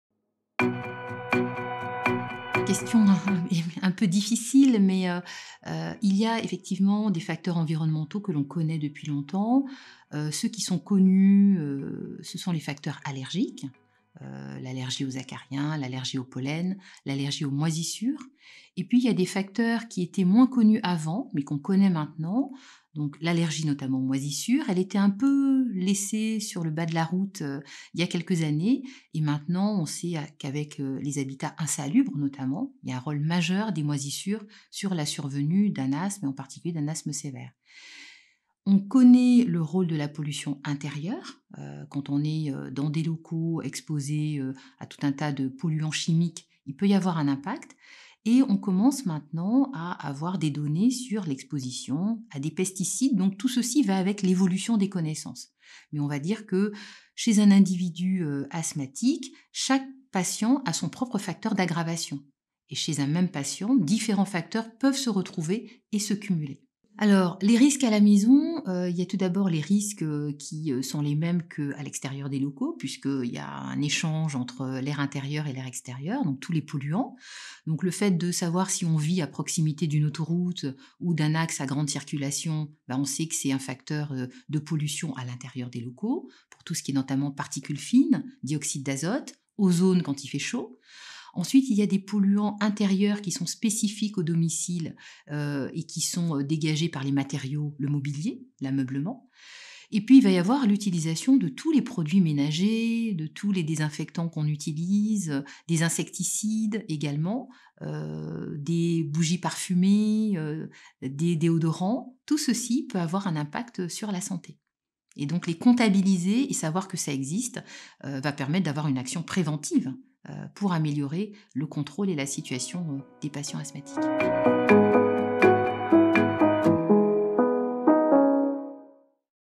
Asthme et facteurs environnementaux
La professeure Chantal Raherison-Semjen, Présidente de la Société de Pneumologie de Langue Française développe dans cette vidéo quels sont les facteurs environnementaux, et notamment les intérieurs, qui sont susceptibles de provoquer de l'asthme.